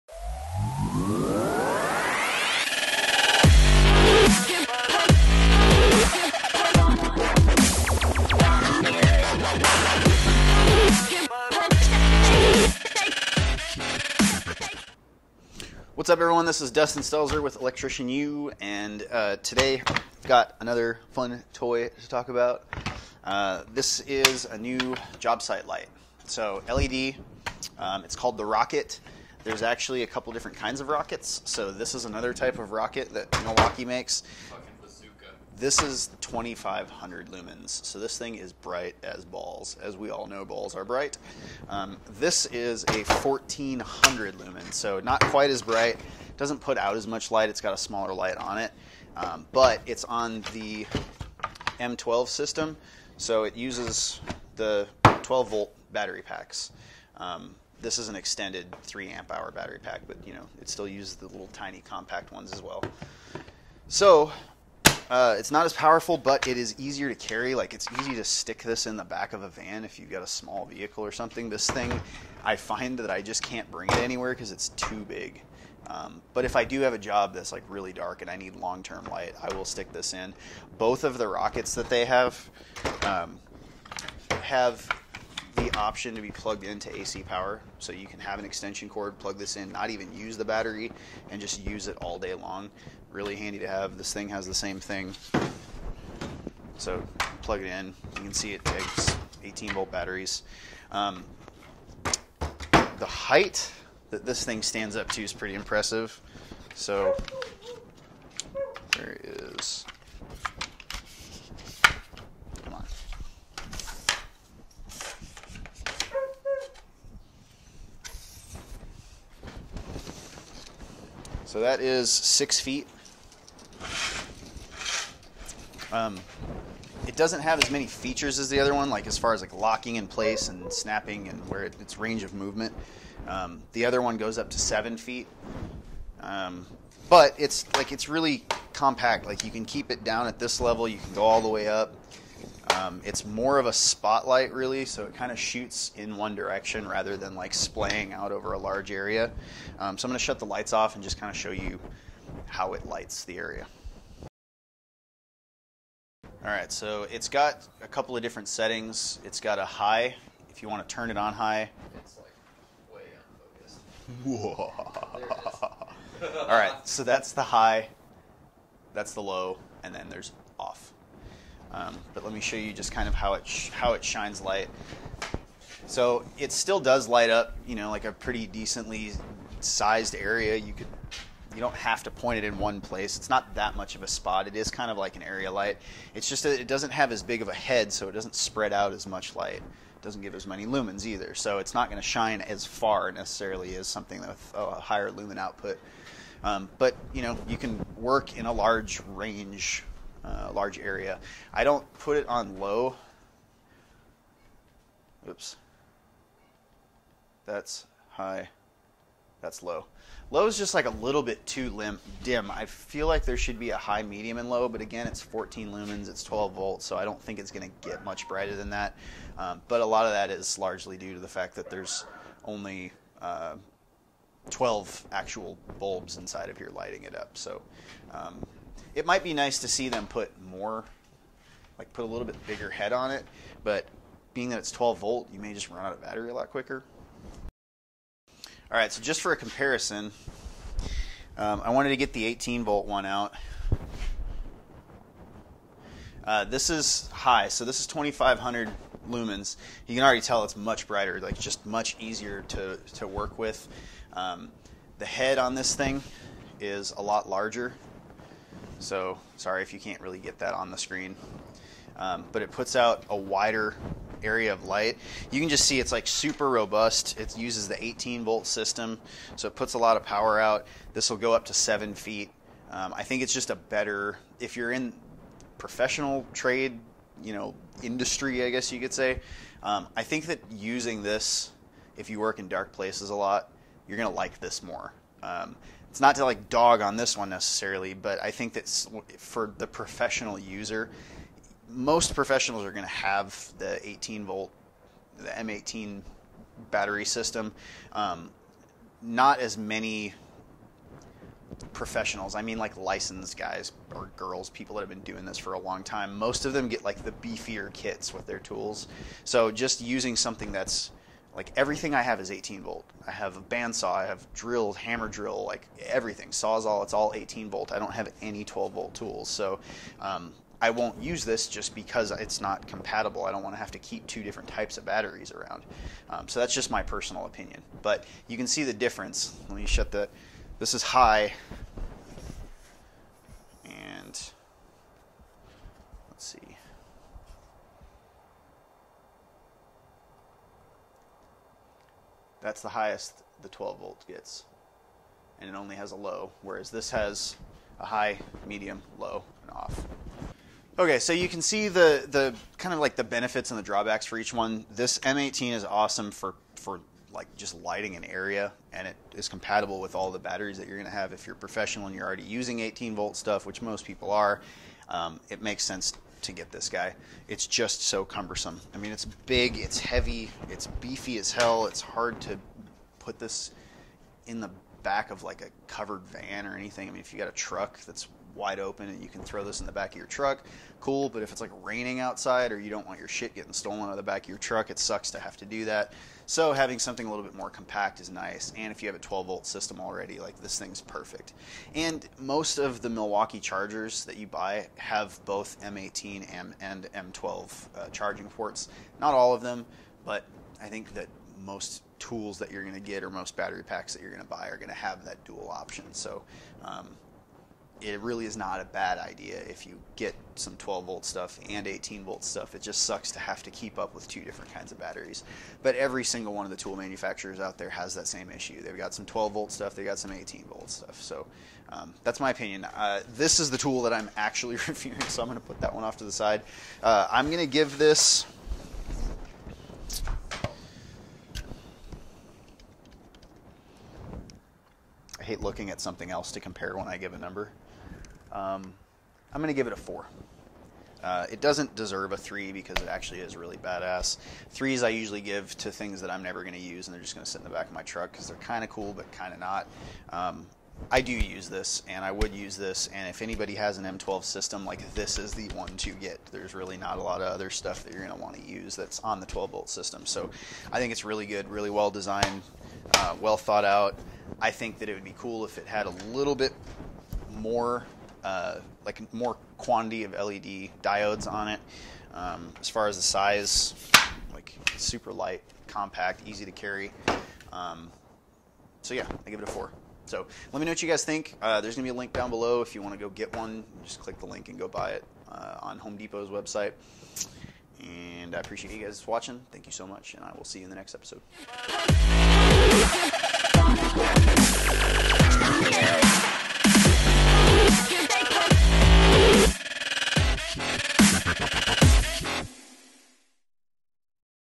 TOOL REVIEW - Milwaukee M12 Rocket 1400 Lumen LED Jobsite Work Light
The Milwaukee M12 Rocket job site light is a very cool new light that I’ve gotten to test out.  I’ve use the larger M18 Rover, and most of the tools I have are M18.  I’ll still use the larger version for sure, but the M12 is much easier to transport and fit into a work truck.  It extends up to 6’ tall, puts out 1400 lumens, and while only having 3 modes (high, low, and off) this little guy packs a punch for it’s size, providing enough light for just about any job you’ll need to do in the dark.

FUNCTIONALITY
This light has telescoping legs that pull out into a tripod, and it stands sturdy whether you’ve got it extended 3’ or out to all 6’.  I personally prefer using the M12 Rocket on jobs because it is much easier to carry around, and it gives me plenty of light to see what I’m doing.

Both the M12 and the M18 have a 110V male receptacle so you can plug it into an extension cord if all your batteries are charging.  This is a killer idea in my opinion, most of Milwaukee’s job sit lights come standard with this feature.

The one flaw i see is that the M18 has several clamping and options that keep the light exactly at the height you want it once you’ve extended and set it to illuminate a certain area.  With the M12, you can only go to the max or minimum height.  If you try to set the neck anywhere in the middle, the head will eventually slide down.  Another thing that I think would make this tool better would be if Milwaukee put more diodes on the head of the light to give you a wider and/or brighter area to work in.

ALSO CHECK OUT THE PODCAST ON ITUNES, SPOTIFY, GOOGLE PLAY MUSIC, IHEARTRADIO, OVERCAST AND STITCHER!